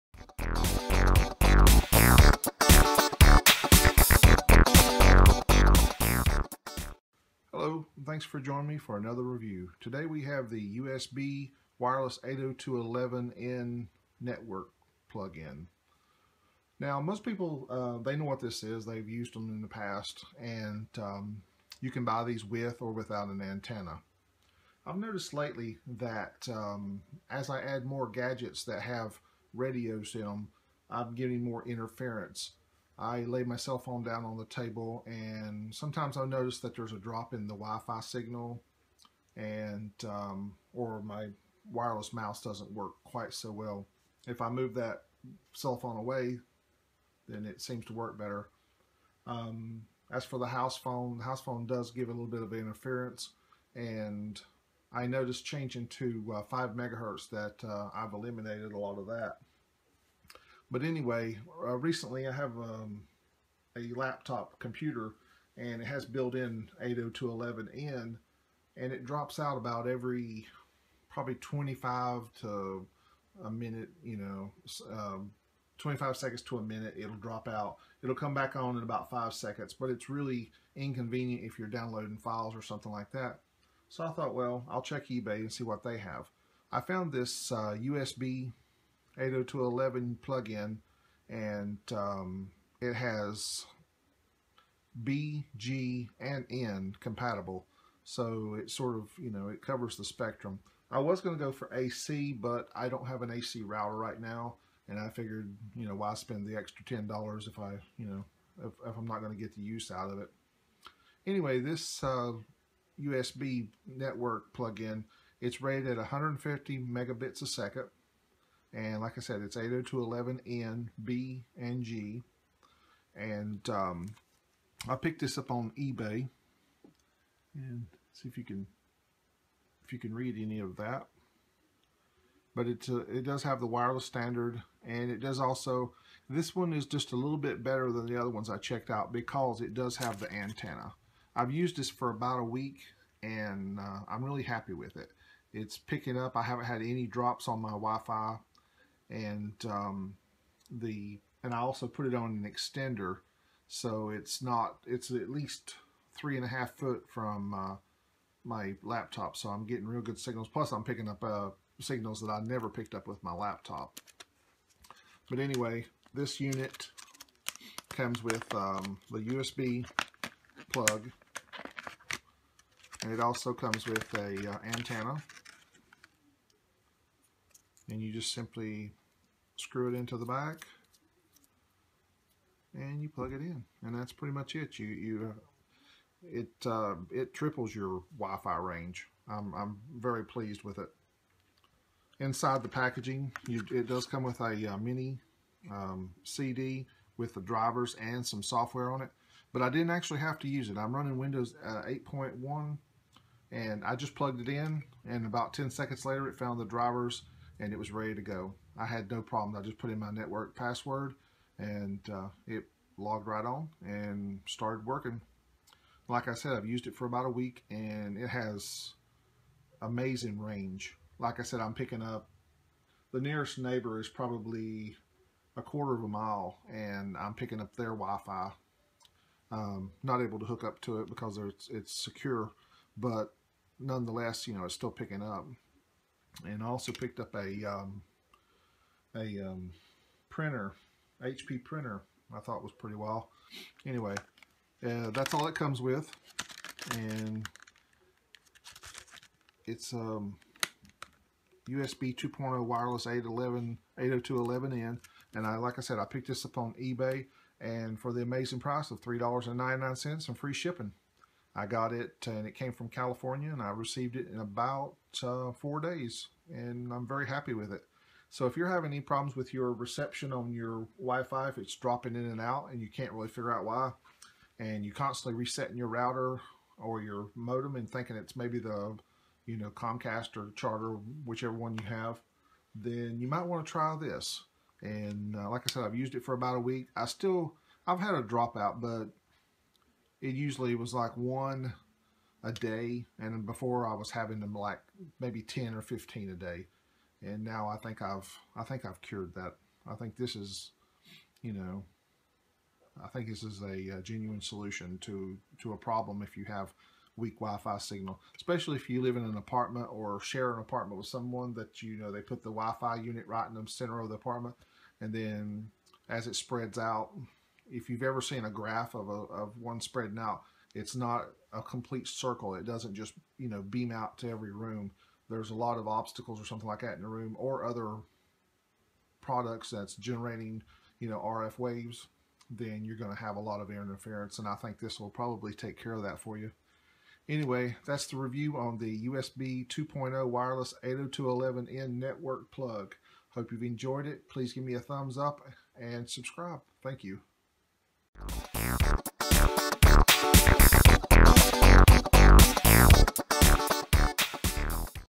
802.11n USB Network Adapter Review
If you have been have trouble with your wifi signal dropping out then you might want to give this a try.
802.11n/g/b USB Network adapter with Antenna.

A lot of the electronics we have transmit RF (Radio Frequency) and can disrupt wifi signals and cause dead zones and drop outs.